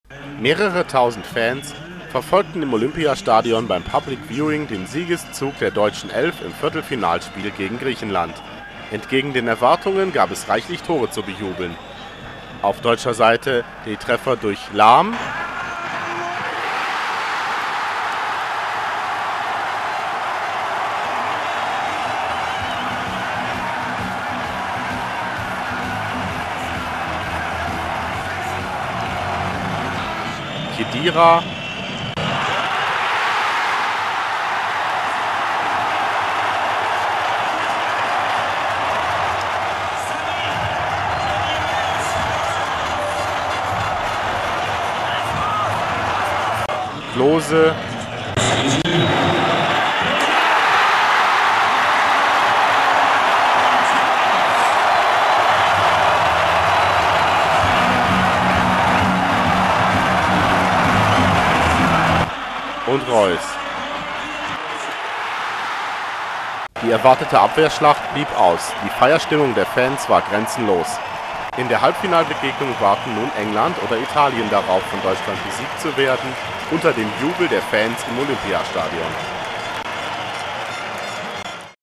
4:2 gegen Griechenland Münchner Fans jubeln!
Tausende Fans feiern Deutsche Nationalmannschaft beim Public Viewing im Olympiastadion